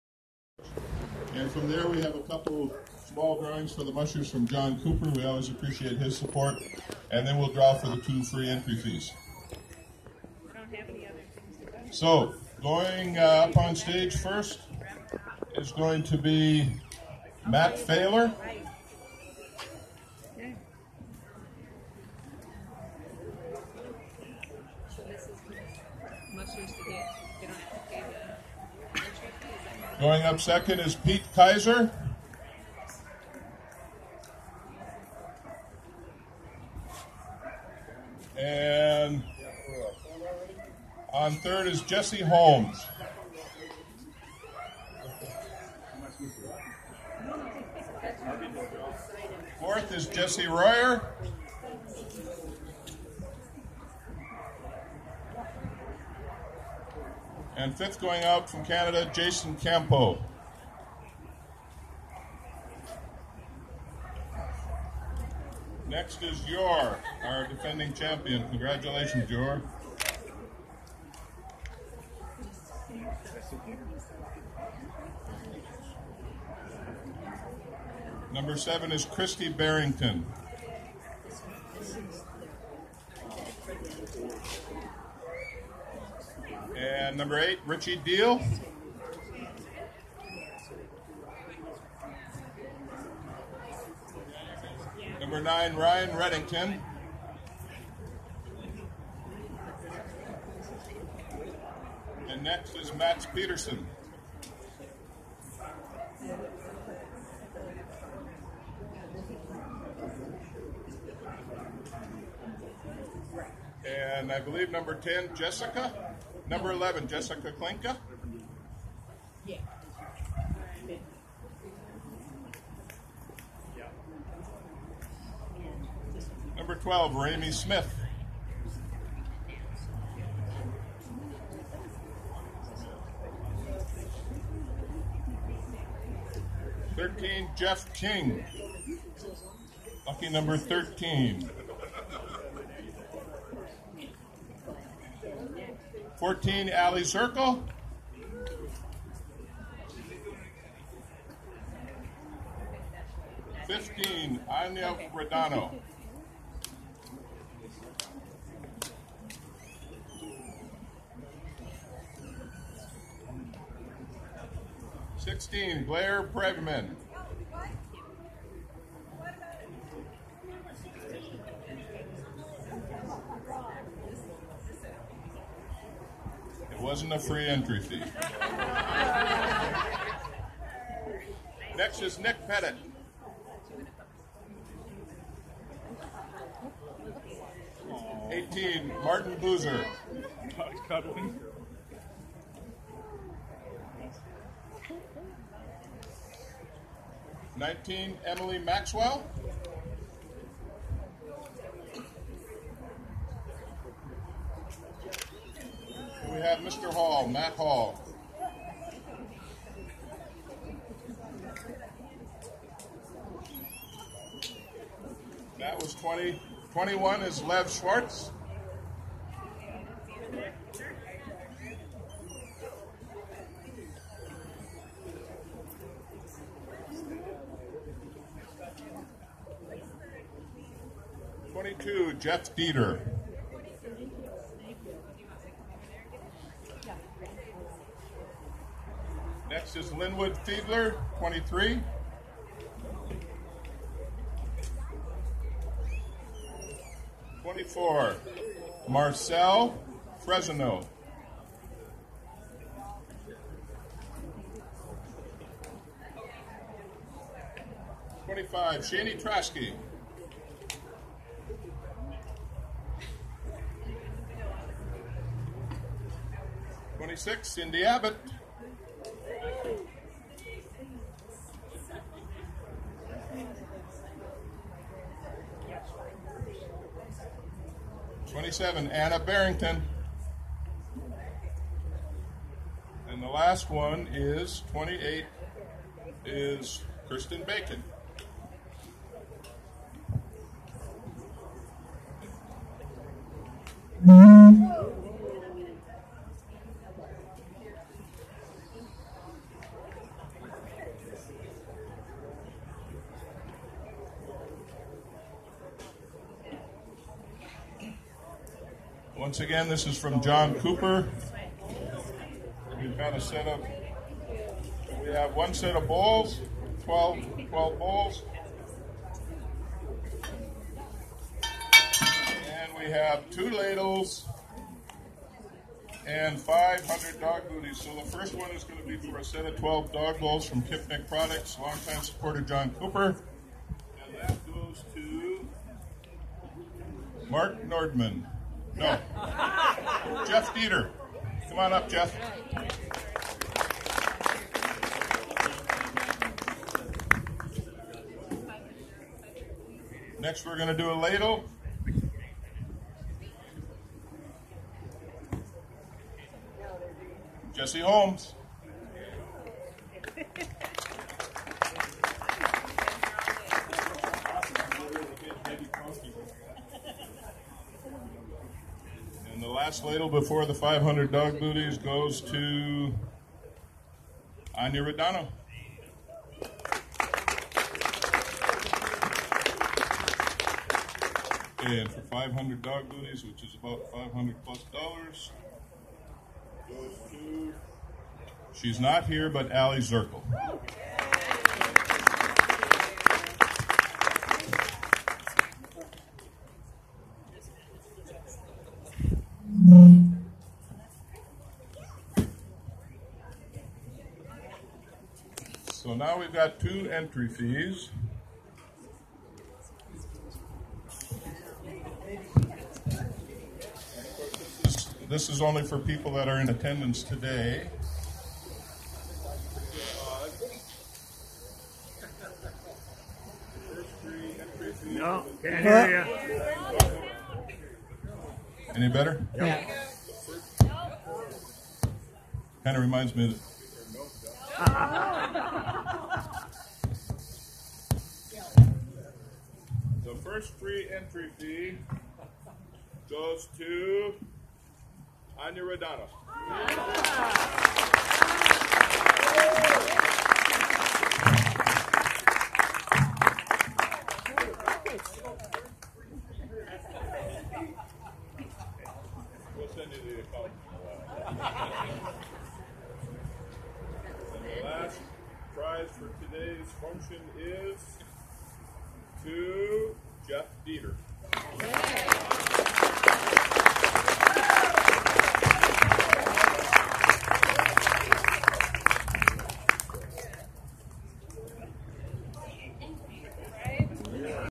Iditarod sled dog ride summer versions
I attended the Iditarod volunteer picnic and took a ride on the cart pulled by a Team of  Raymie Redington's sled dogs